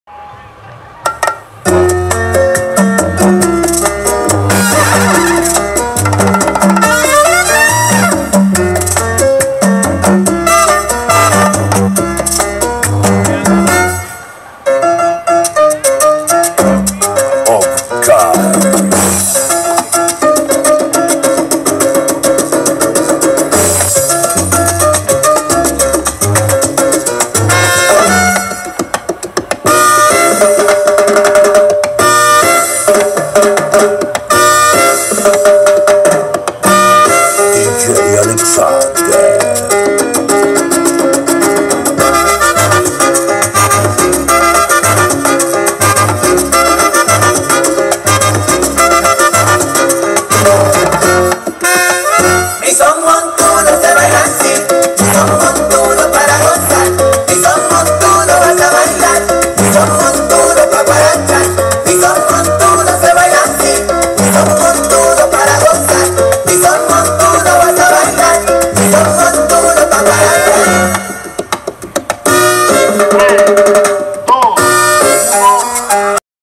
7ma Valida Nacional OPCAR 2018 / Lecherias Tour Infinity DSC 0455
Evento que se llevo a cabo Lecheria en las Instalaciones del CC Plaza Mayor el pasado 28 y 29 de Septiembre

& TUNING

Recuerden Difundir